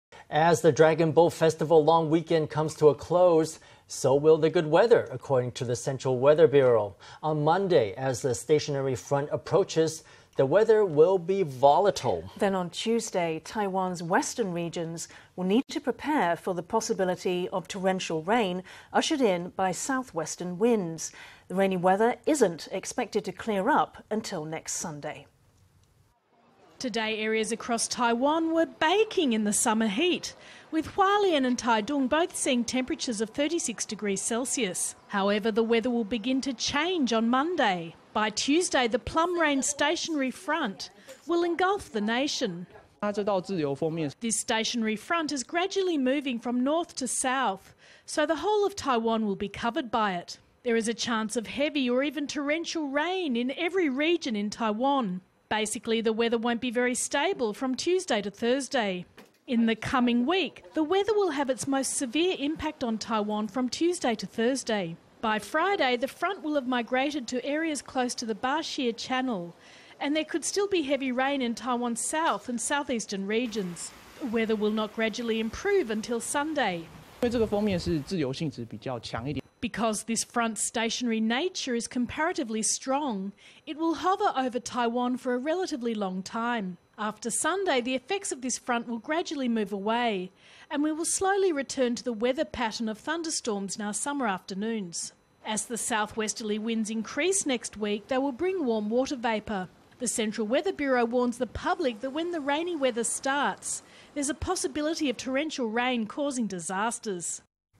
Forecasters say there could be torrential rain this week under the influence of a stationa...
As the Dragon Boat Festival long weekend comes to a close, so will the good weather, according to the Central Weather Bureau. On Monday, as a stationary front approaches, the weather will be volatile. Then on Tuesday Taiwan''s western regions will need to prepare for the possibility of torrential rain ushered in by southwestern winds. The rainy weather isn''t expected to clear up until next Sunday.  Today areas across Taiwan were baking in the summer heat, with Hualien and Taitung both seeing temperatures of 36 degrees Celsius. However, the weather will begin to change on Monday. By Tuesday, the Plum Rain stationary front will engulf the nation. Kuan Hsin-pingCWB ForecasterThis stationary front is gradually moving from north to south, so the whole of Taiwan will be covered by it. There is a chance of heavy or even torrential rain in every region in Taiwan. Basically the weather won''t be very stable from Tuesday to Thursday. In the coming week, the weather will have its most severe impact on Taiwan from Tuesday to Thursday. By Friday, the front will have migrated to areas close to the Bashi Channel and there could still be heavy rain in Taiwan''s south and southeastern regions. The weather will not gradually improve until Sunday. Kuan Hsin-pingCWB ForecasterBecause this front''s stationary nature is comparatively strong, it will hover over Taiwan for a relatively long time. After Sunday, the effects of this front will gradually move away and we will slowly return to the weather pattern of thunderstorms in our summer afternoons. As the southwesterly winds increase next week, they will bring warm water vapor. The Central Weather Bureau warns the pubic that when the rainy weather starts, there''s a possibility of torrential rain causing disasters.